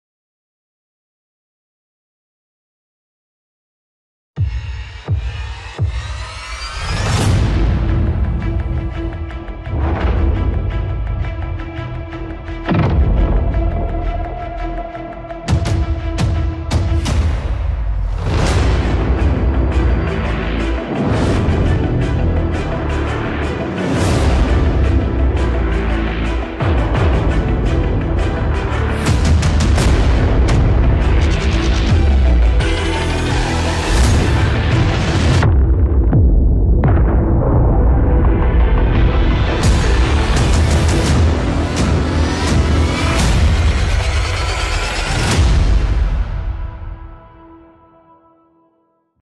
HESTIA '22 - Official Aftermovie [ Trailer ] | TKM College of Engineering | Techno-Cultural Fest
Get ready to be transported back to a world of pulsating beats and dazzling sights, as we bring to you the after movie of Hestia'22 which took our college by storm! For four unforgettable days, we celebrated the spirit of tomorrow with a lineup of sensational acts and breathtaking performances. The wait is over, so join us in reliving the magic and experiencing the infinite memories that we once made.